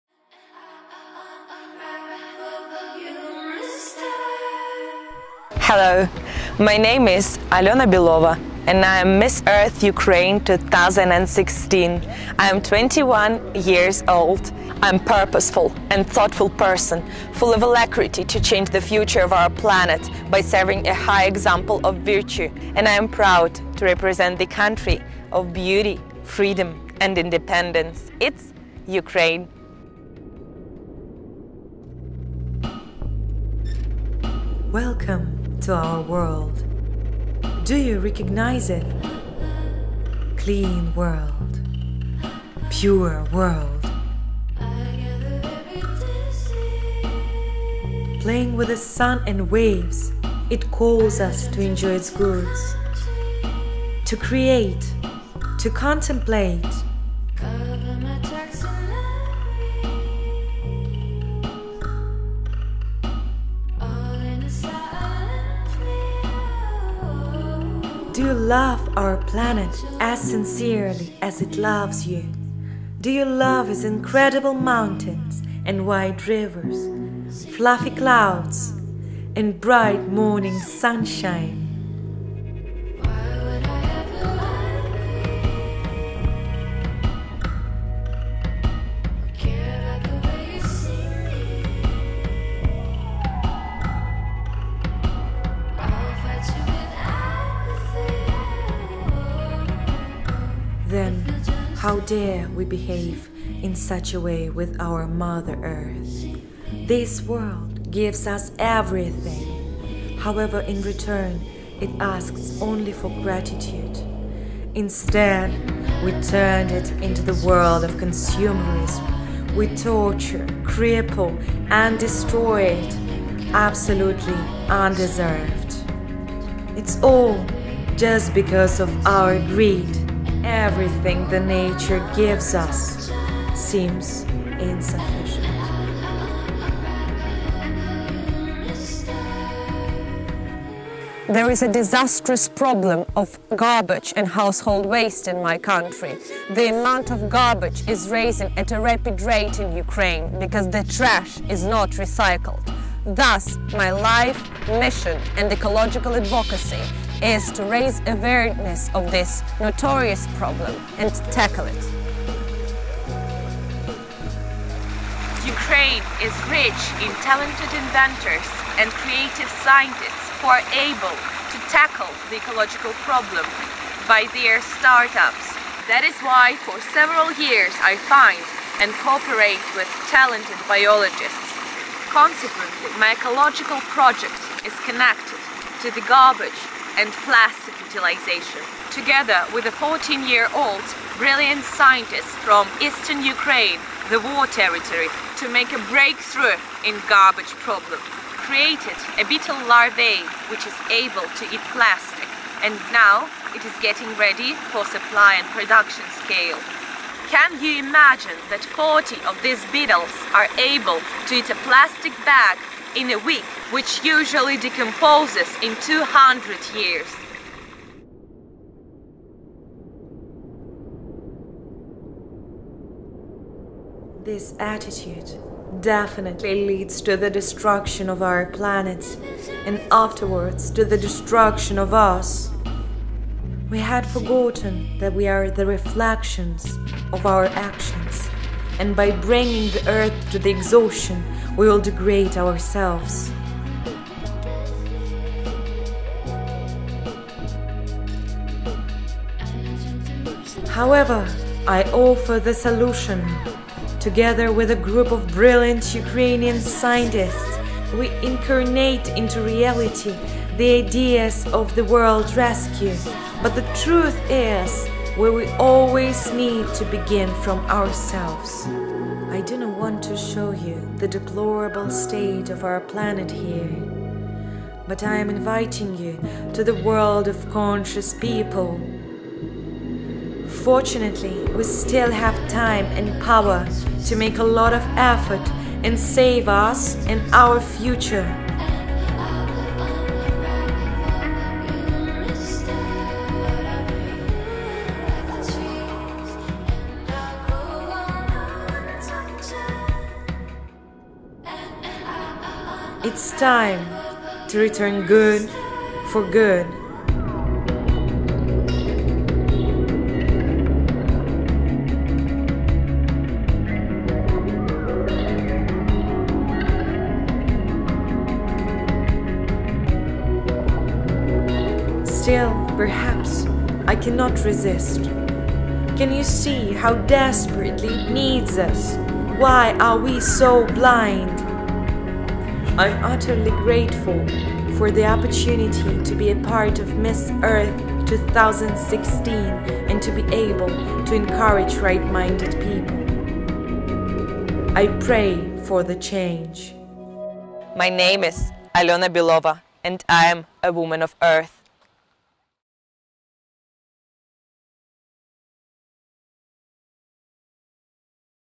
Eco Beauty Video Miss Earth Ukraine Alona Bielova
Эко-видео Miss Earth Ukraine-2016 Алены Беловой для международного конкурса Miss Earth.
Режиссер: Иван Кваша
Одежда: Julia Aysina, RUTA